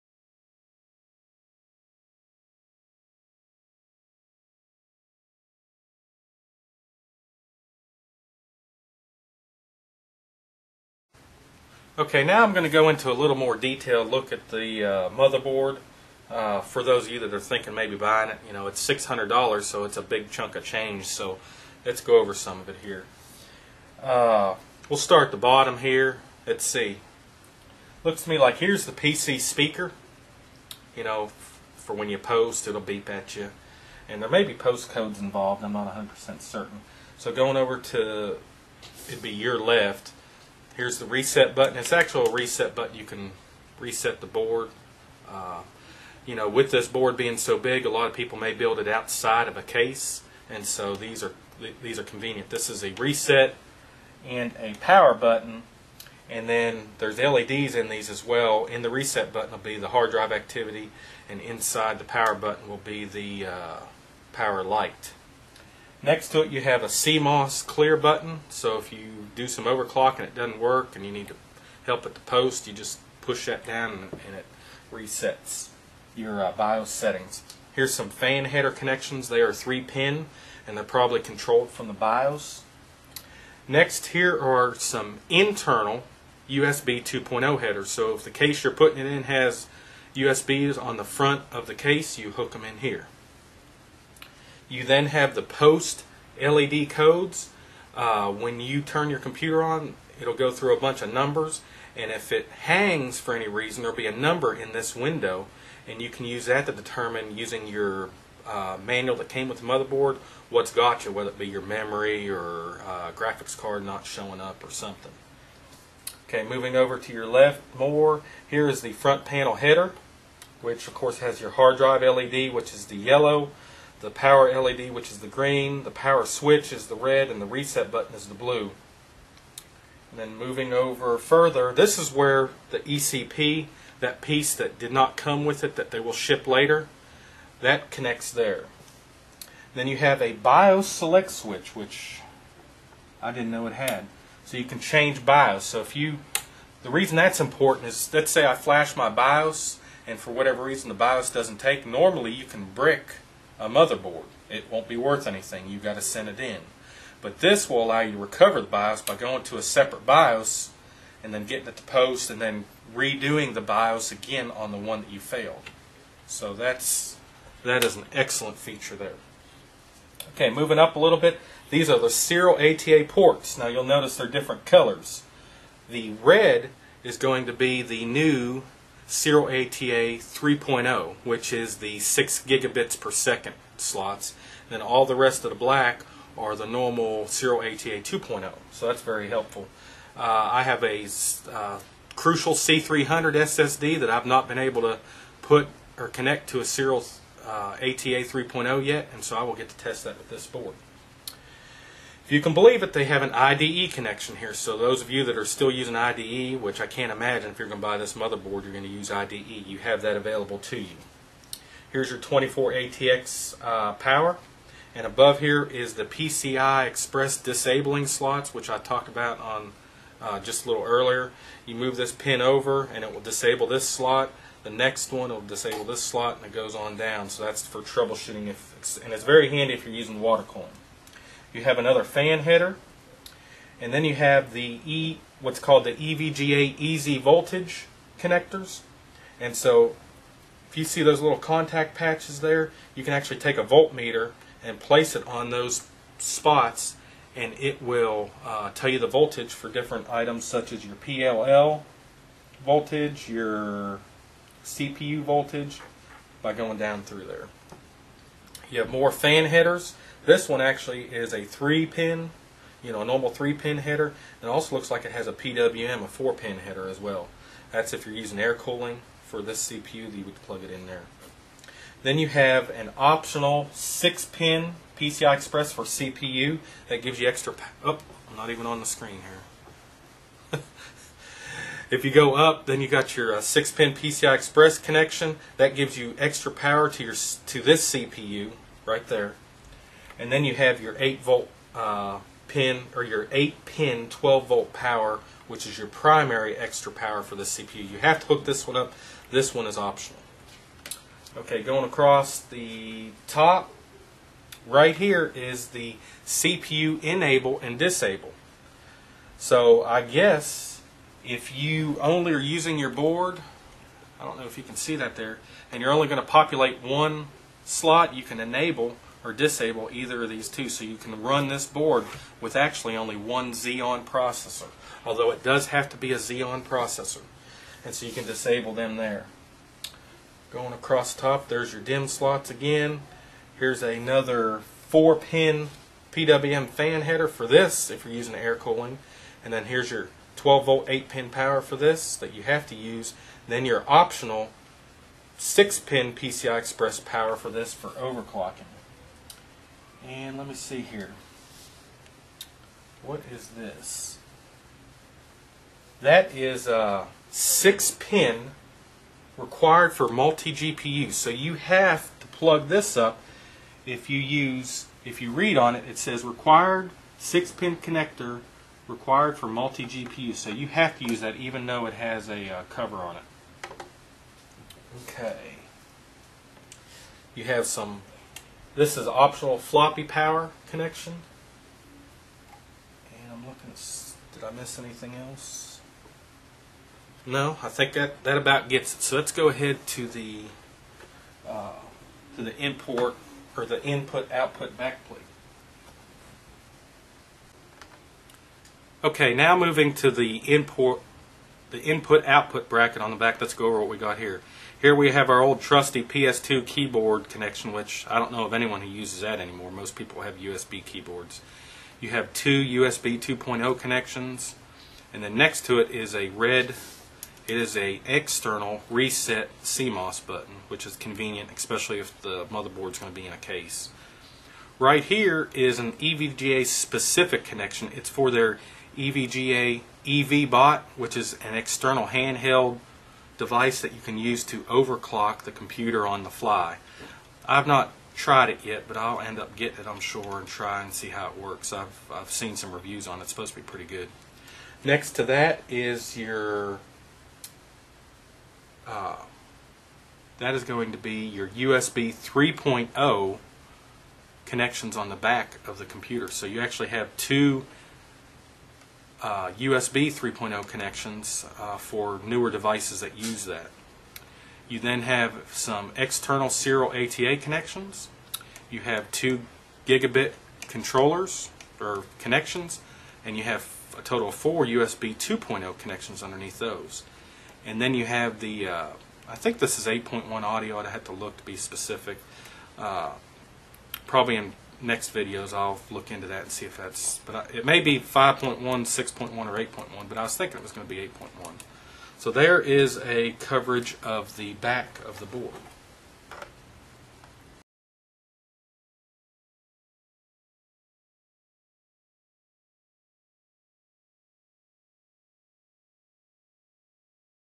EVGA Classified SR-2 Dual XEON Motherboard Detailed Overview / Features (270-WS-W555-A1)
Now let's take a closer look at the EVGA SR-2 HPTX Motherboard.  I go over the entire motherboard and try to point out all the features that I notice on the motherboard --in case anyone is thinking on spending the $600 for it.

It has a huge amount of features including SATA III & USB 3.0!  There are also some EVGA only features as well.